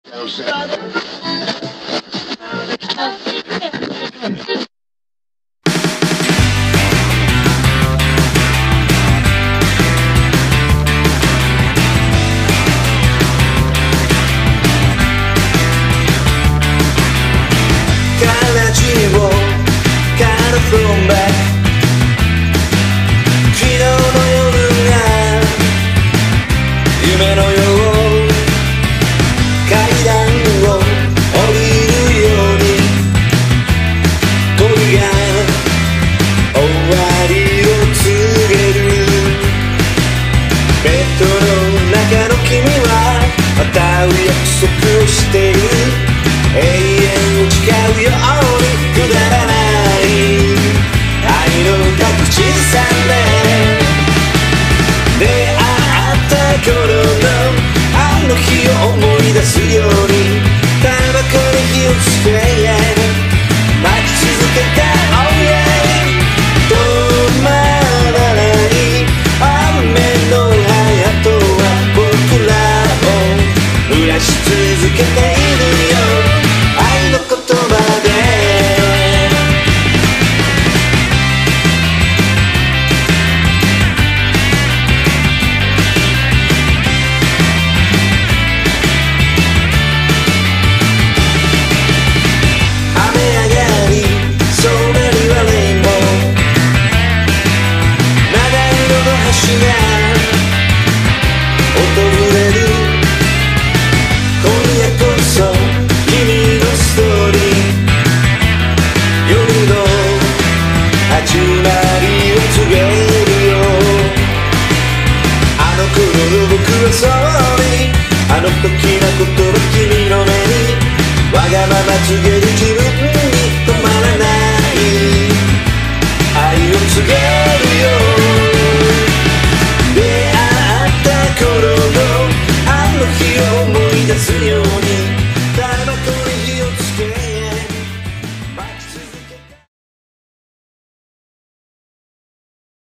[CHOPPERS TV]WEST BAR FUKUI OPEN PARTY IPPUTASAI
北陸福井にWEST BARがオープン
１００名収容のBARです